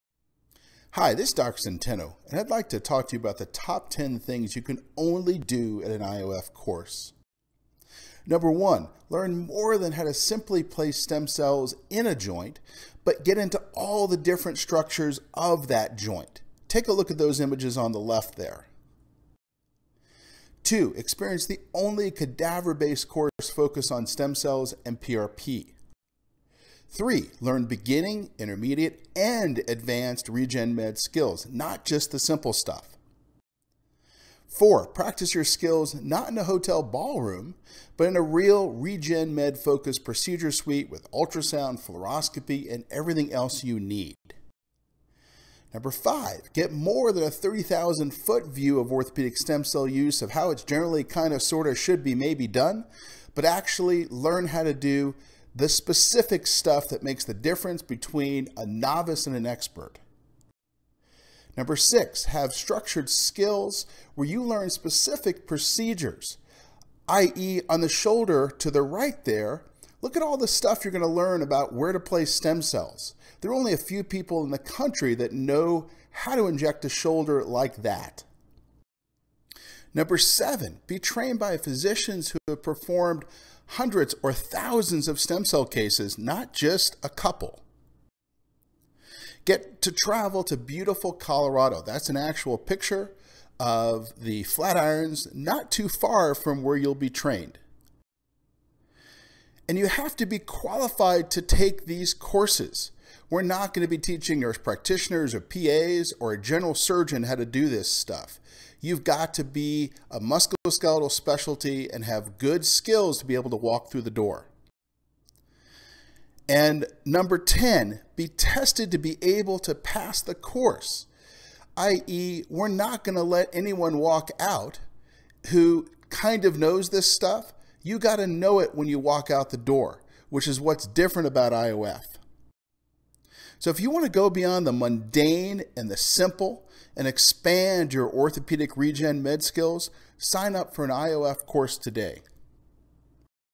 Top 10 Things You Can Only Do at an IOF Course
Dr. Centeno describes what makes the Interventional Orthopedics Foundation courses so different from other regen med courses teaching stem cells and PRP.